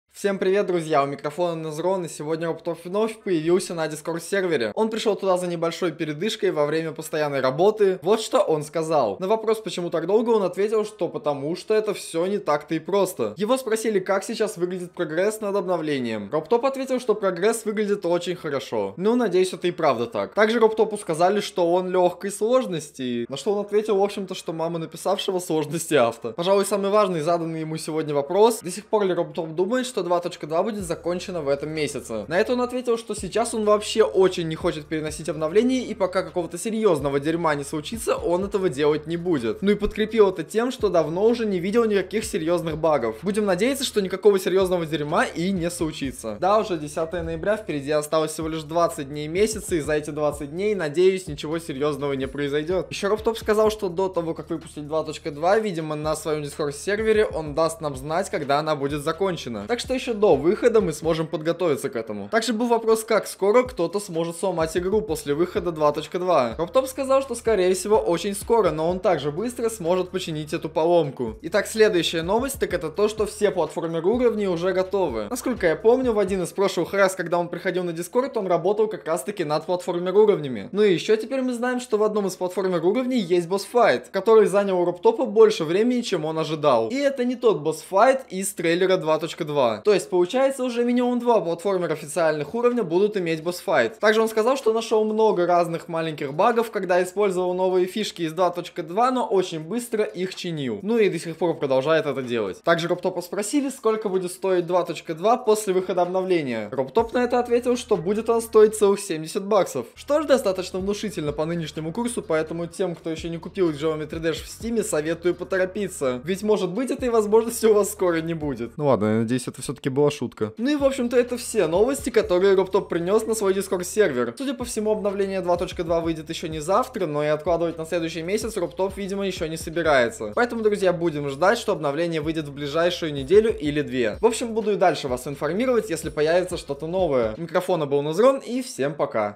РОБТОП ПЕРЕНЕС 2.2 НА ДЕКАБРЬ? GEOMETRY DASH NEWS
00:00 Вступление
00:09 Почему так долго?
00:13 Прогресс обновления
00:20 Робтоп легкой сложности
00:26 Перенос на декабрь?
00:52 О выходе 2.2
01:02 Слом игры
01:11 Все платформеры готовы
01:34 Много багов
01:42 ГД будет стоить 6к
02:00 Конец